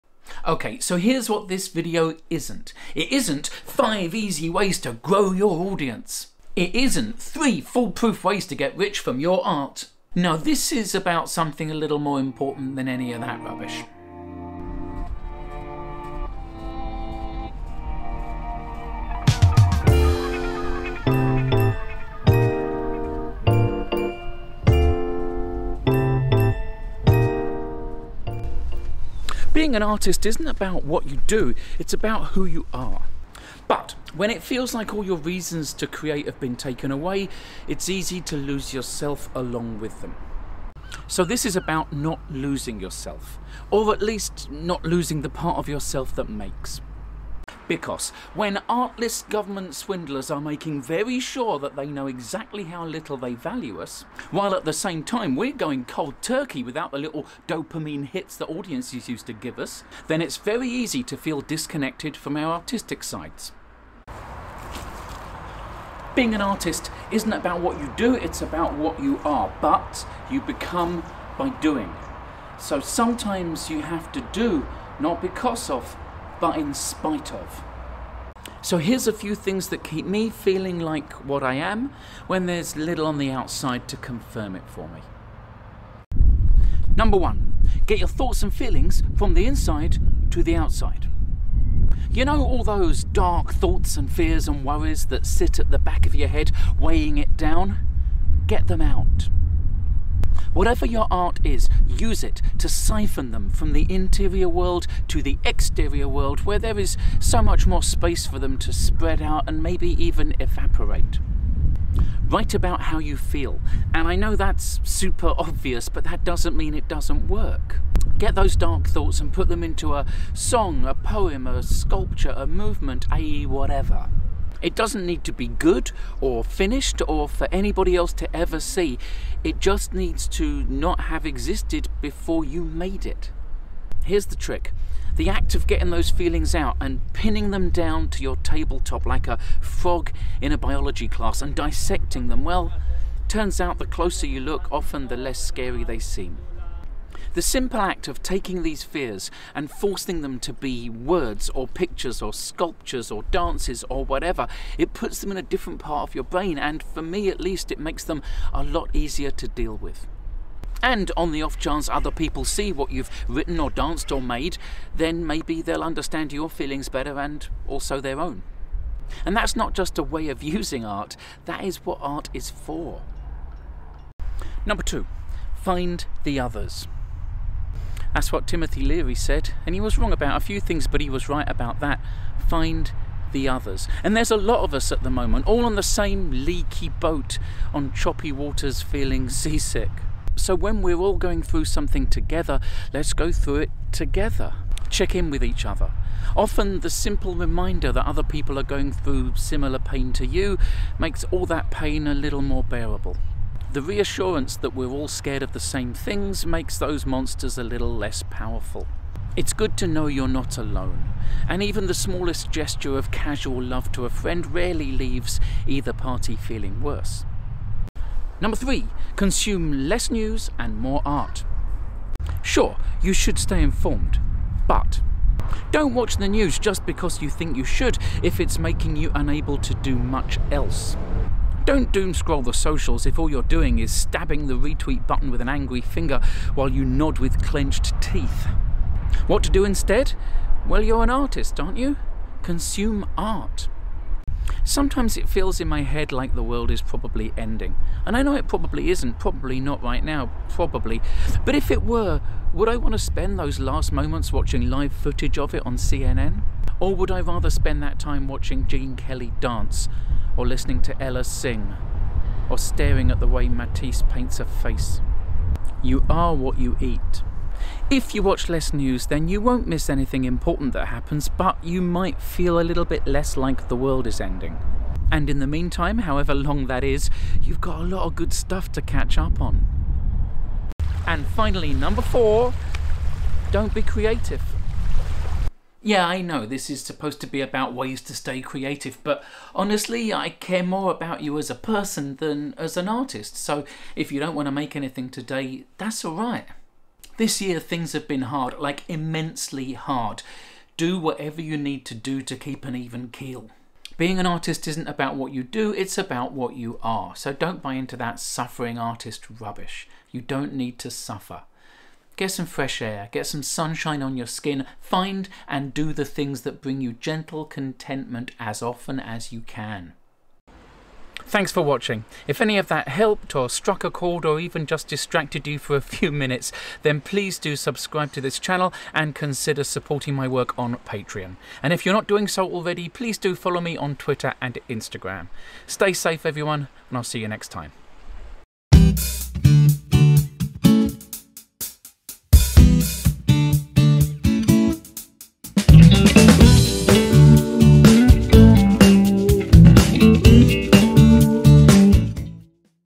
4 ways to stay artistic in tough times
It's a tough time for independent artists and makers at the moment, so I thought I'd share a few things that help me stay feeling like..what I am...